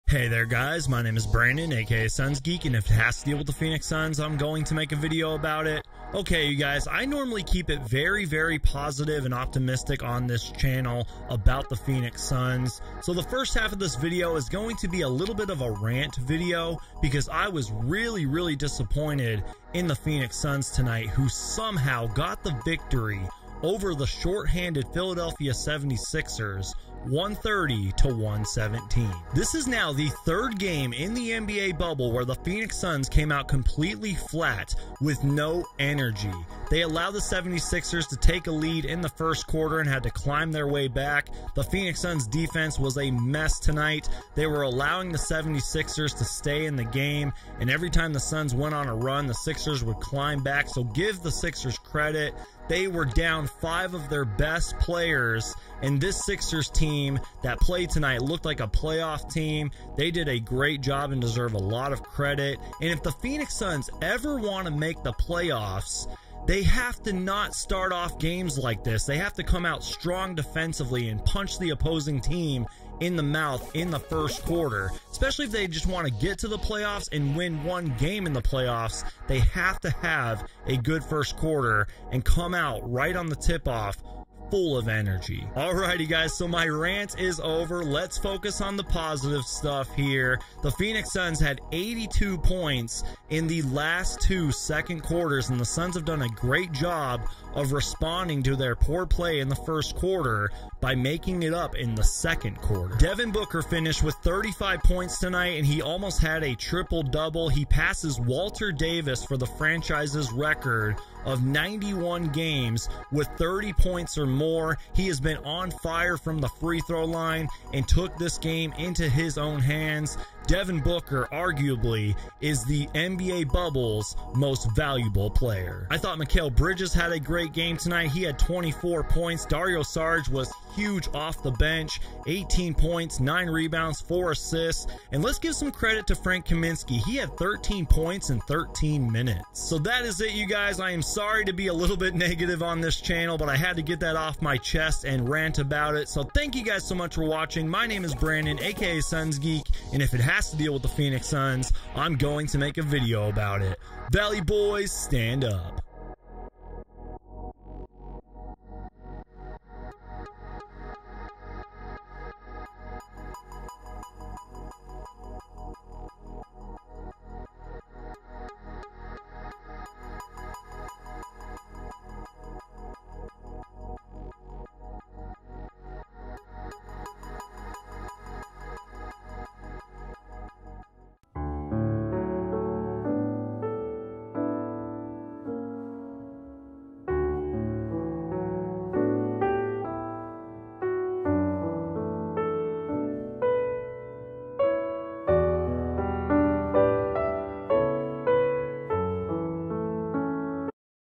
Phoenix Suns Stay Undefeated With UGLY Win Over Sixers (Recap/Rant)
Phoenix Suns get an UGLY win over the short handed Philadelphia 76ers tonight, in the first half of this video i rant about why i was disappointed in the Suns tonight then focus on the bright spots with highlights and stats!